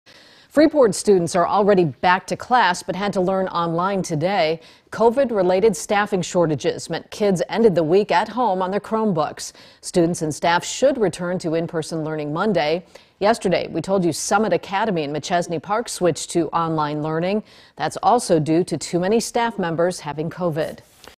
Freeport School District announces remote learning Friday due to COVID-related staff shortages
The Freeport School District announced Friday that all of their schools will be taking an adaptive pause from in person learning for the day due to COVID-related staff shortages.